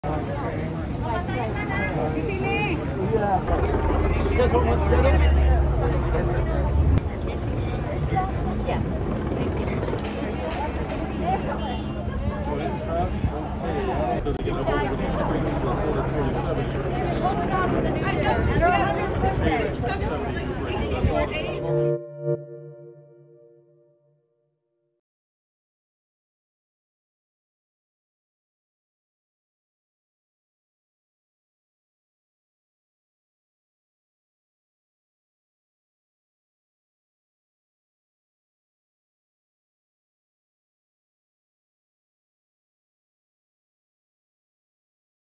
Fans Gather at Betty White's Hollywood Walk of Fame Star
Fans adorned Betty White’s Hollywood Walk of Fame star with flowers and a copy of her book after it was announced that the “Golden Girls” star died Friday morning.